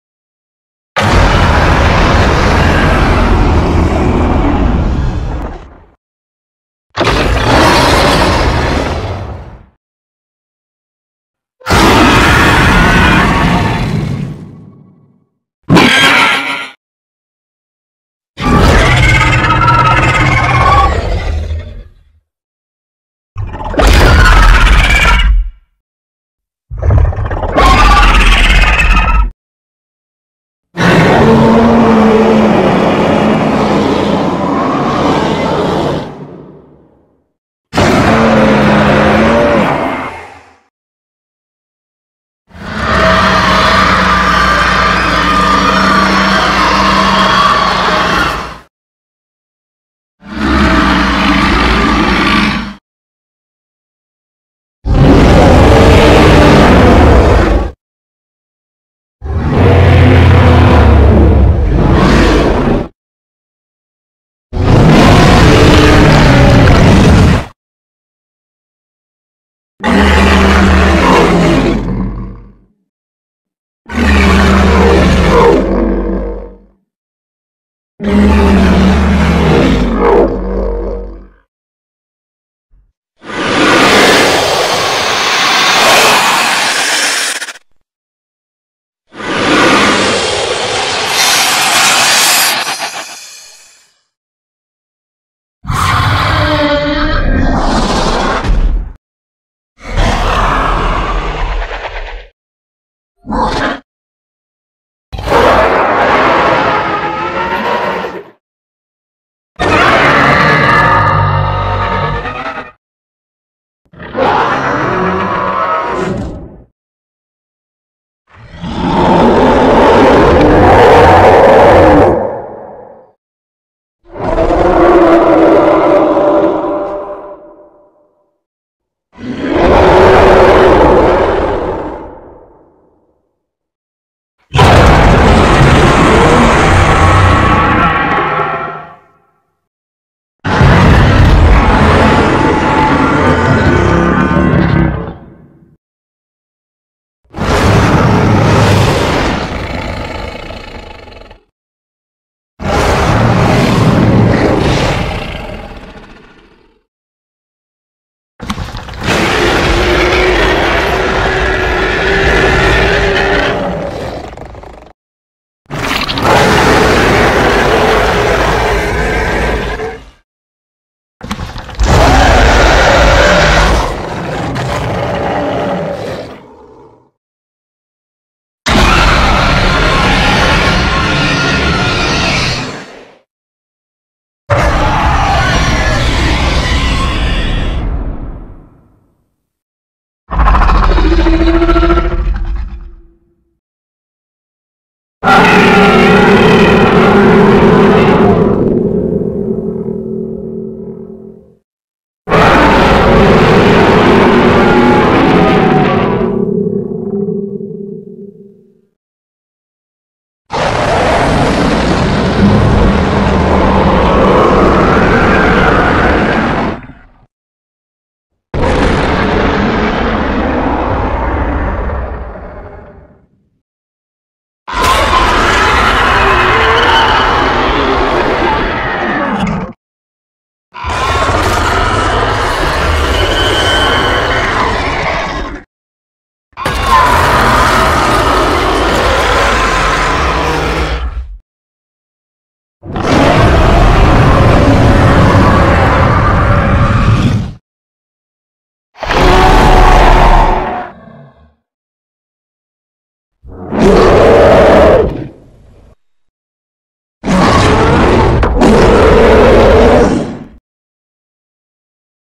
Weathering With You 2: Destroy All Creatures Sounds Part 1 (OUTDATED)
Weathering With You is back for a second movie of Battle Of Creatures! More characters! More puns! More MAKOTO SHINKAI!

Sound Of Creatures:
0:00 Titanus Yujin Ōzora
0:11 Titanus Blue
0:17 Legendary Bezelb
0:30 Titanus Ice Bear
0:42 Titanus Swampy
0:54 Titanus Manny
1:09 Titanus Stegomon
1:22 Anime Buraki
1:34 Titanus Heimlich & 1:44 Titanus Mr Dinosaur
1:57 Titanus Irina Shido
2:14 Titanus Rossweisse
2:26 Titanus Greymon
2:37 Titanus Barney
2:57 Titanus Vail Lucifer
3:14 Mecha Delta Bowser/Mecha Gamera
3:30 Titanus Rockwell
3:43 Titanus Unknown Hybrid Bird
4:01 Titanus Vertigo/Buraki’s Dad
4:12 Titanus Natsumi Suga